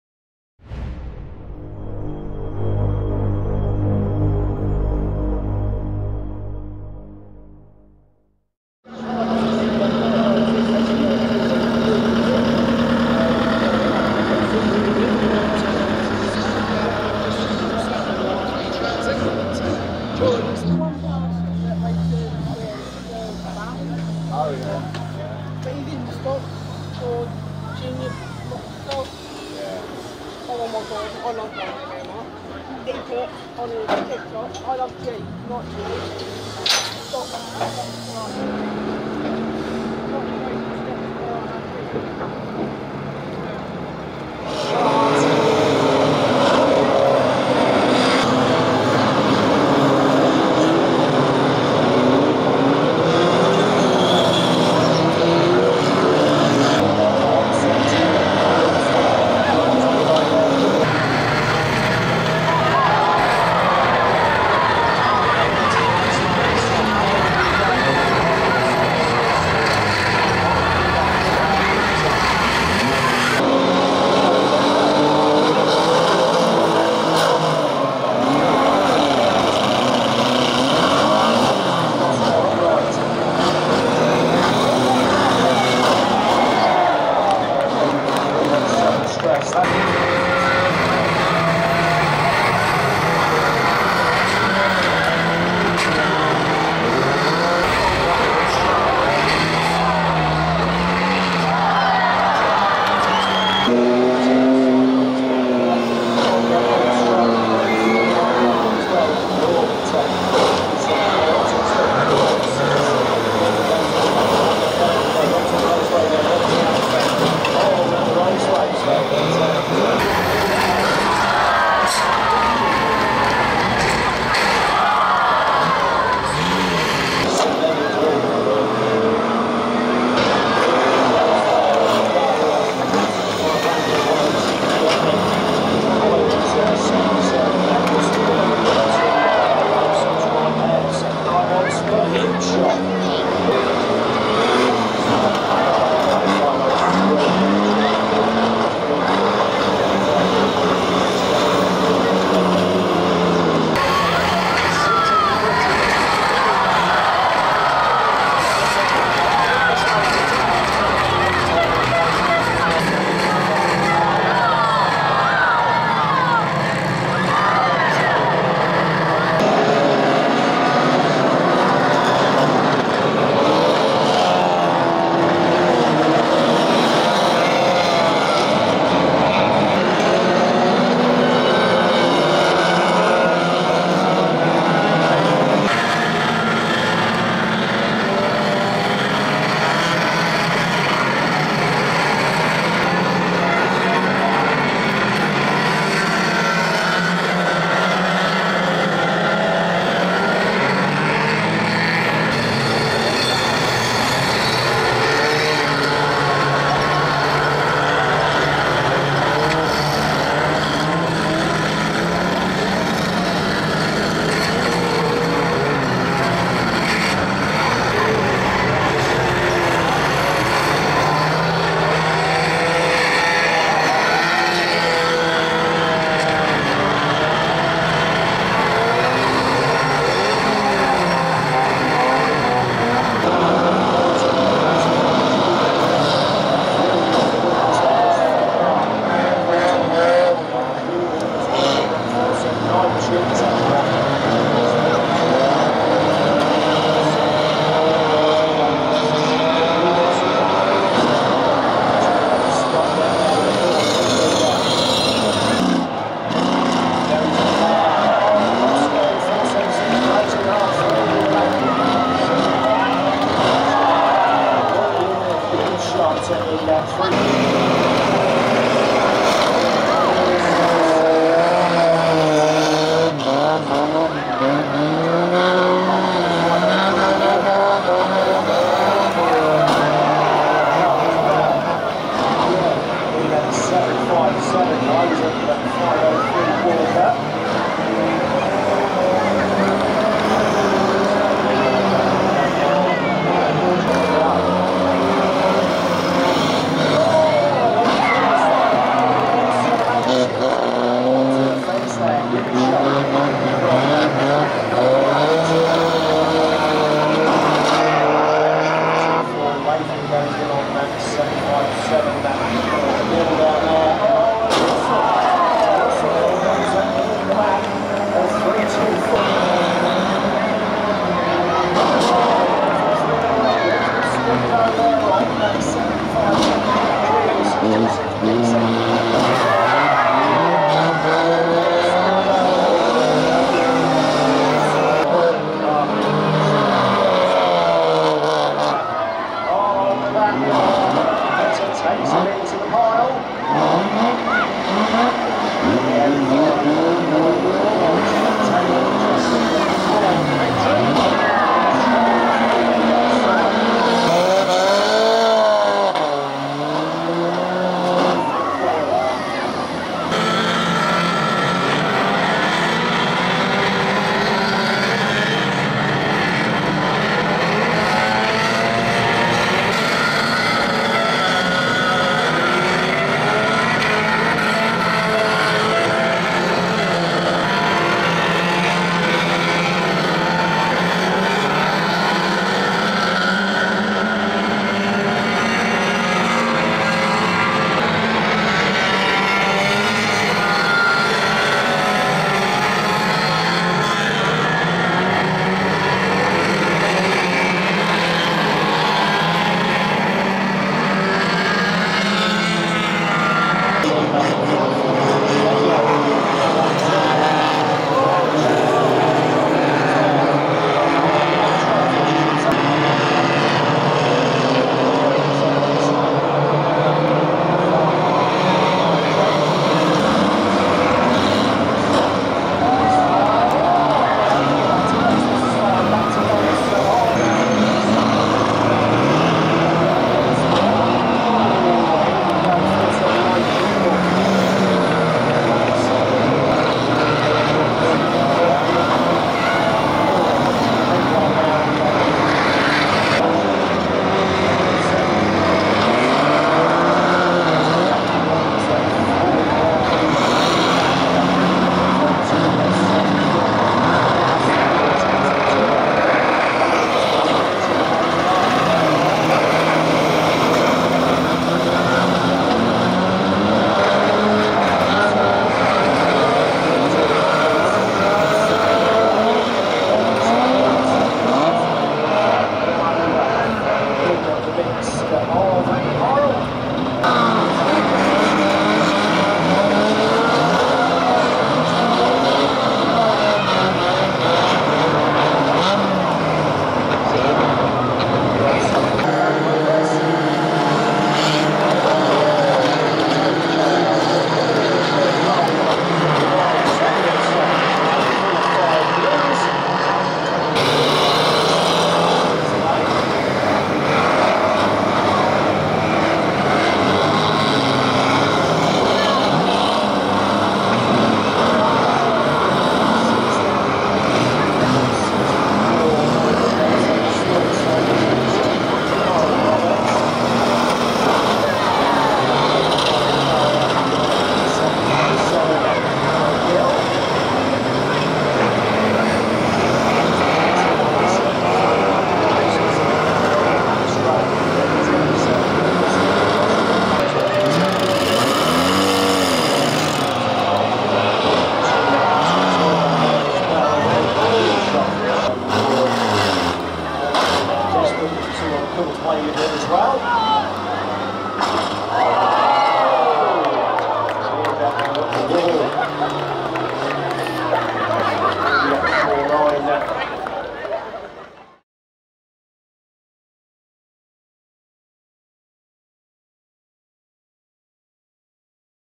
Kings Lynn Van Banger Racing Mayhem for MND Highlights Aug 2023
A selection of highlights from the August Bank Holiday Monday 2023 Big Van Bangers Charity day. Raising funds for motor neurone disease. Also featuring some crash and rollover clips from the 1500cc Bangers and Reliant Robins. Some big vans and big action! The full version features multi camera edit of the races, action replays, pit and wreck shots and some driver interviews too.